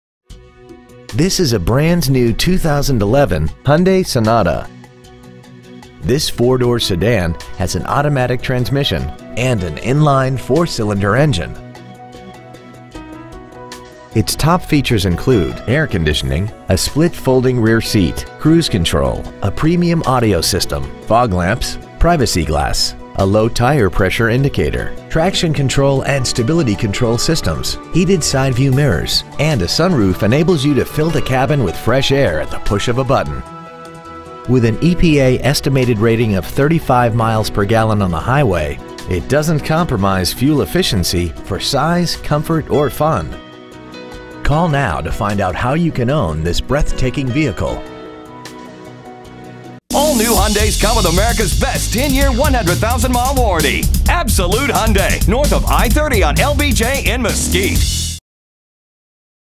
2011 Hyundai Sonata Mesquite TX
We've been honored to serve the Mesquite TX area , we promise that your experience at our dealership will exceed your expectations !

 Year : 2011
 Make : Hyundai
 Model : Sonata
 Engine : 2.4L 4 Cyl. Fuel Injected
 Trans . : Automatic
 Exterior : Harbor_gray
 Miles : 281

 Stock : BH236763

 2011 Hyundai Sonata Mesquite TX